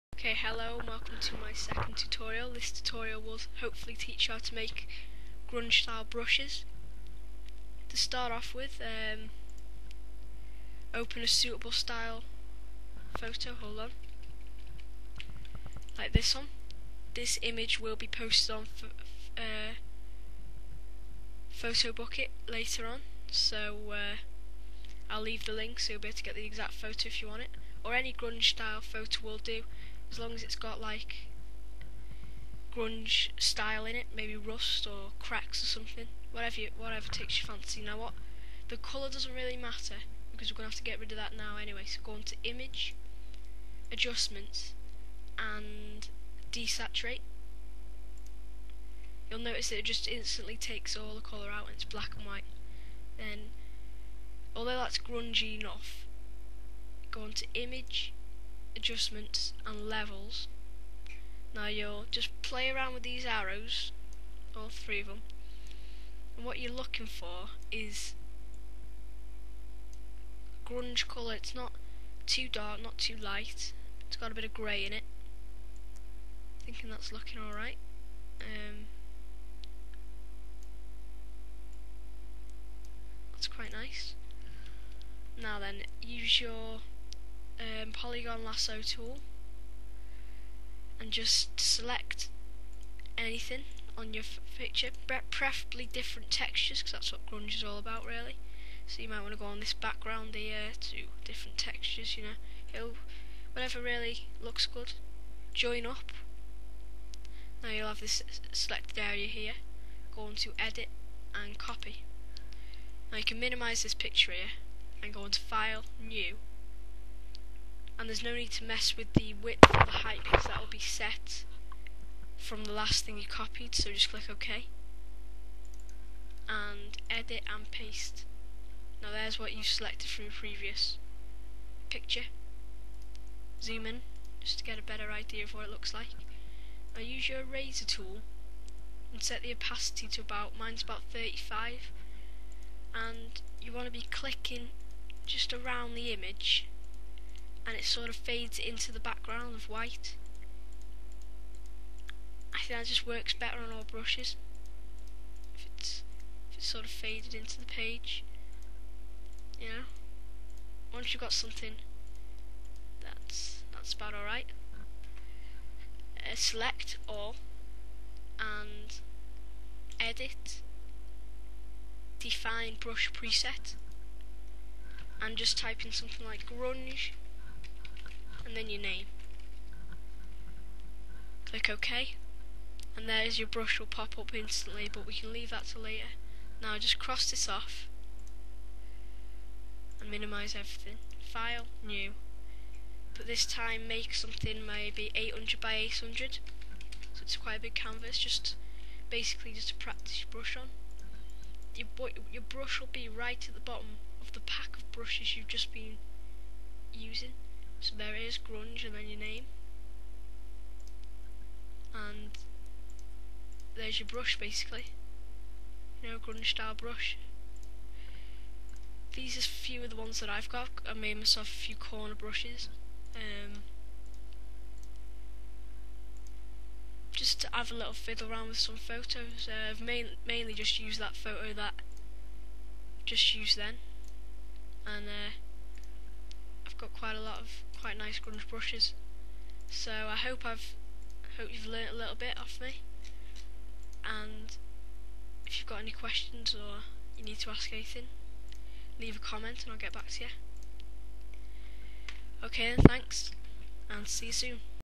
How to make a grunge style brush using Photoshop.
This is my tutorial and its quite a basic easy tutorial.  The image i used to make the brush is found here:

 Please comment if you need anything or if you want to tell/ask me something. Thanks